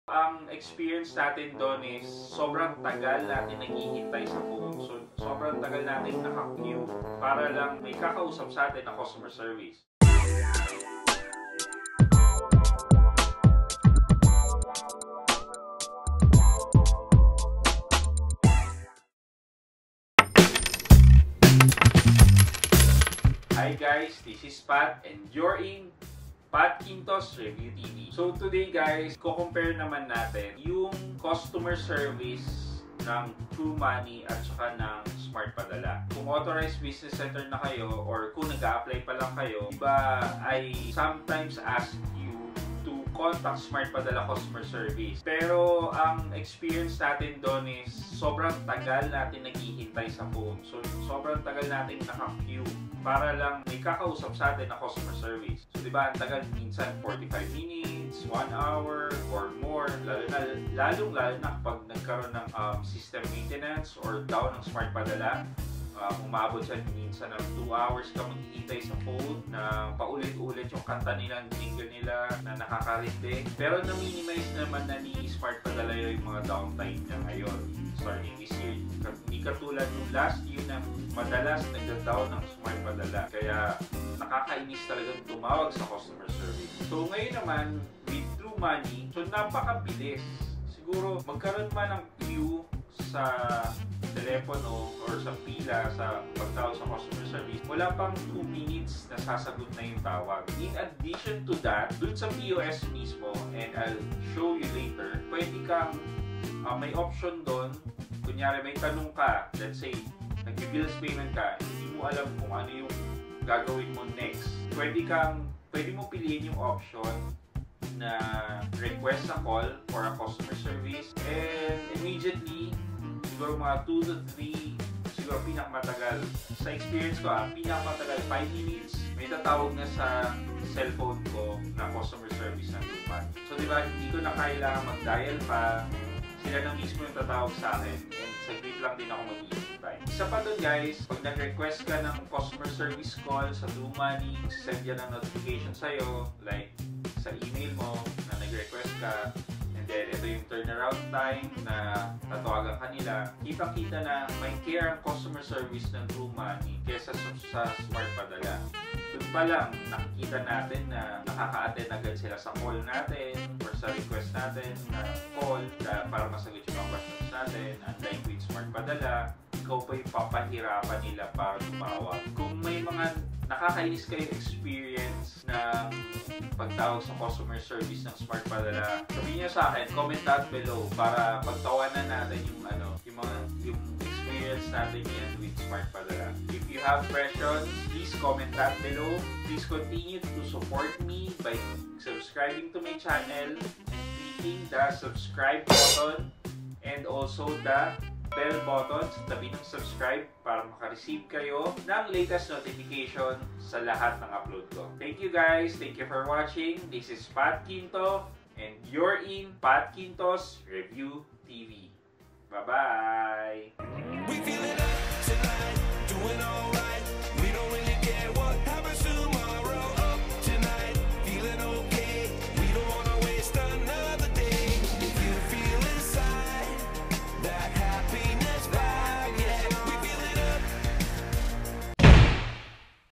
Worst Customer Service Ever?!! - Smart Padala- VLOG #57
VLog #57 - This video provides comparison between Smart Padala and True Money's customer service.